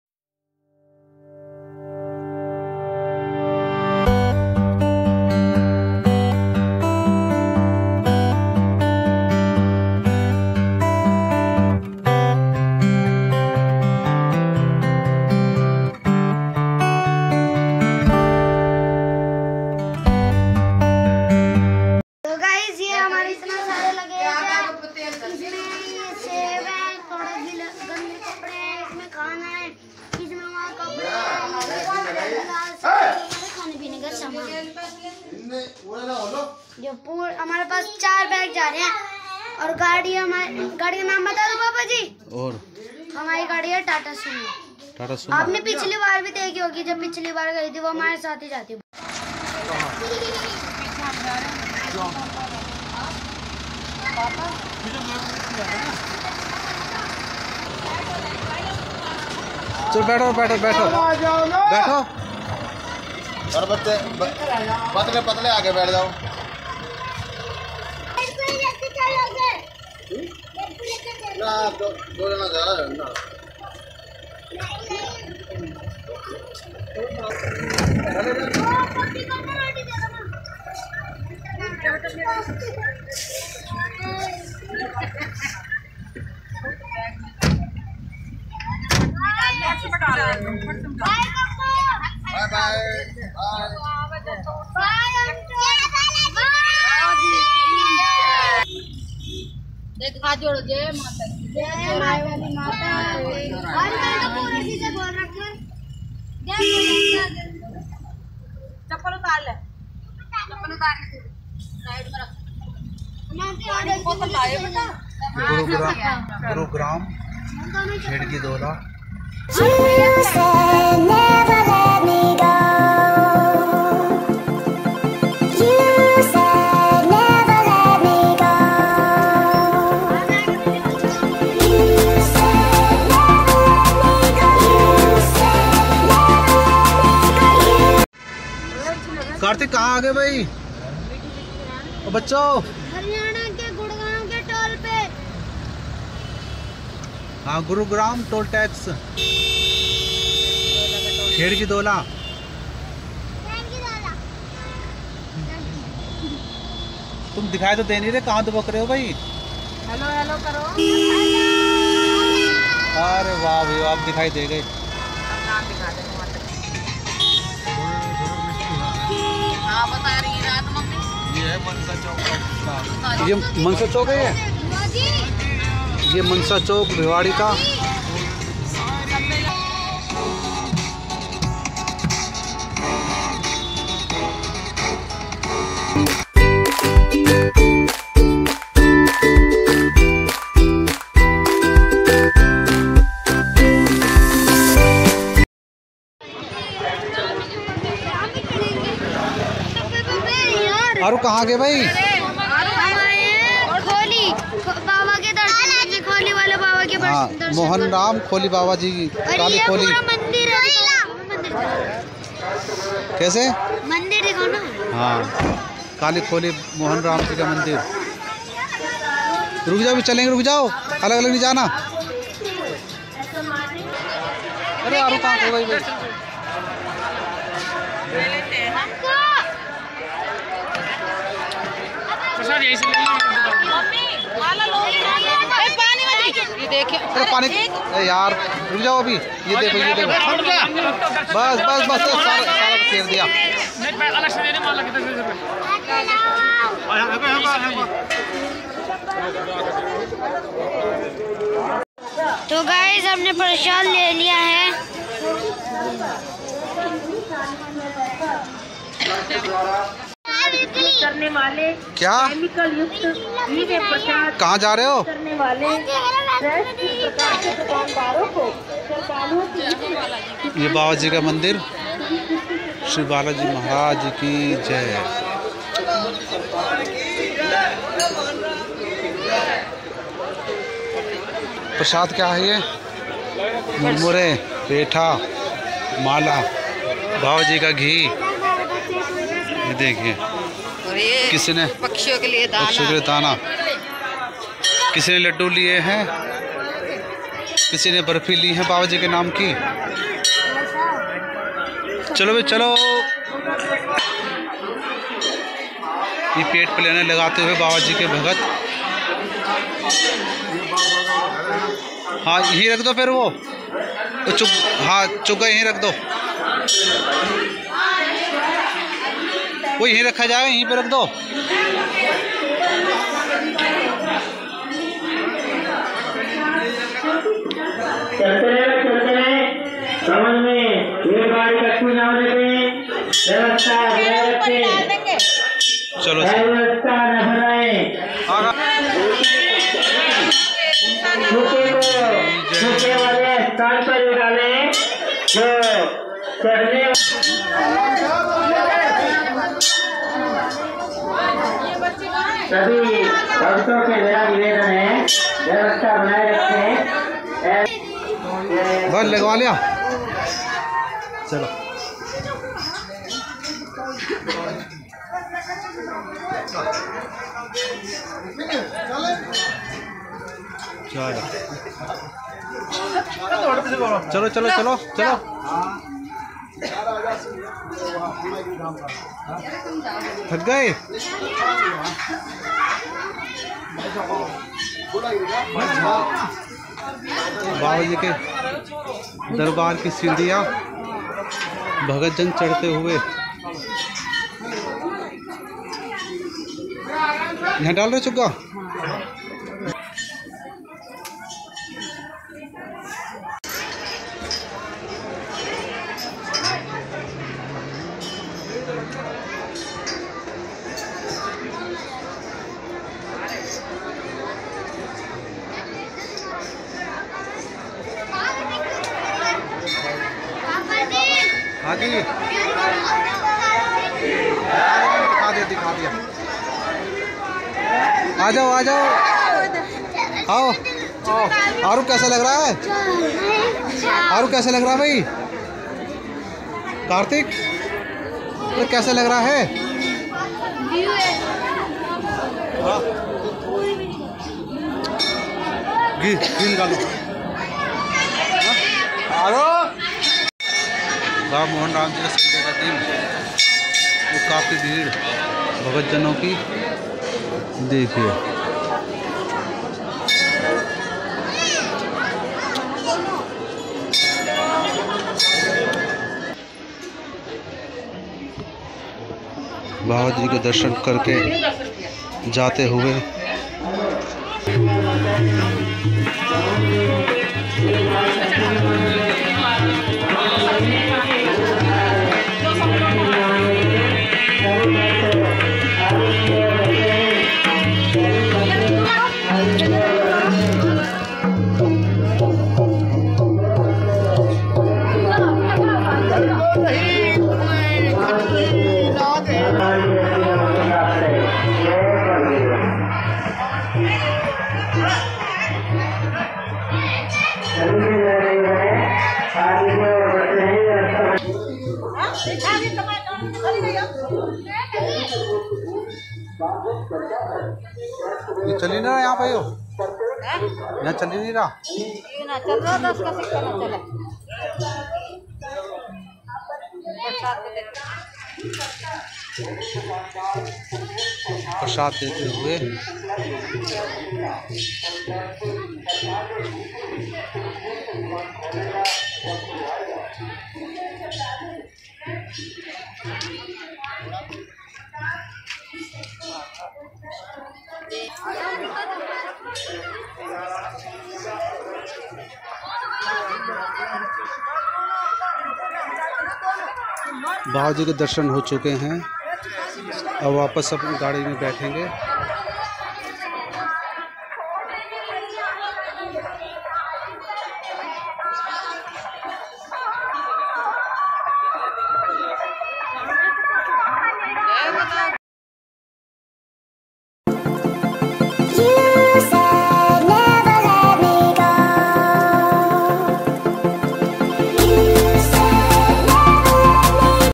Kali Kholi dham yatra .please like subscribe 🙏🙂
Kali kholi dham yatra.ye kali kholi dham rajasthan me hai Jo mohan baba kali kholi naam se bhi Jana jata hai or yahan Sree Krishan ka hi rup virajman hai jai ho baba mohan ram ji ki